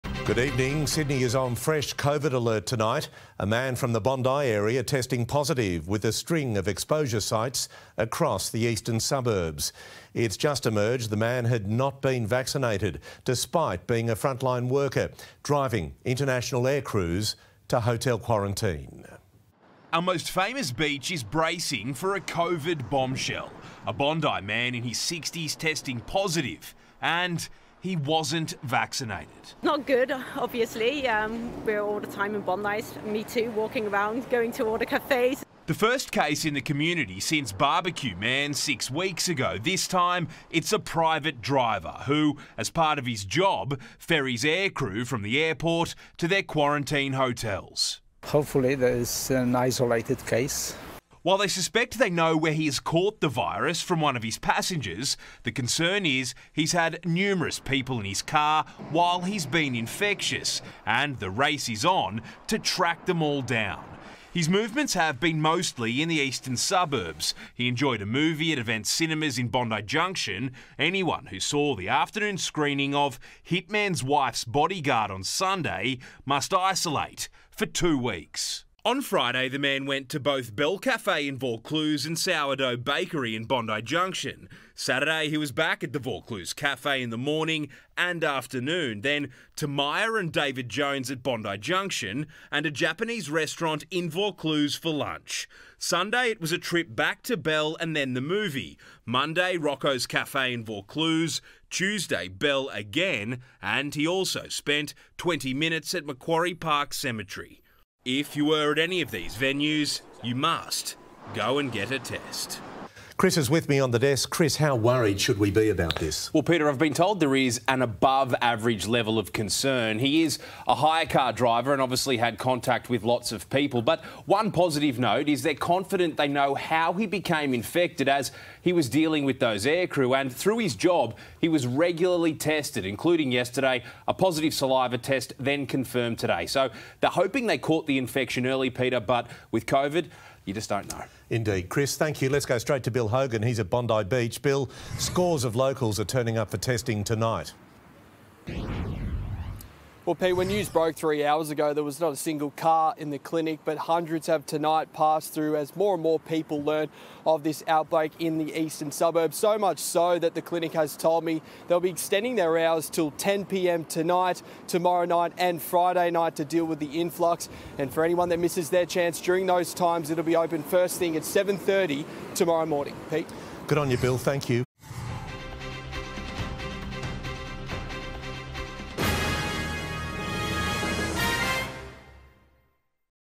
Bondi COVID-19 case under investigation | Coronavirus | 9 News Australia
A new case of coronavirus has been detected in Bondi, in Sydney's Eastern suburbs and has been identified as a driver for international flight crews.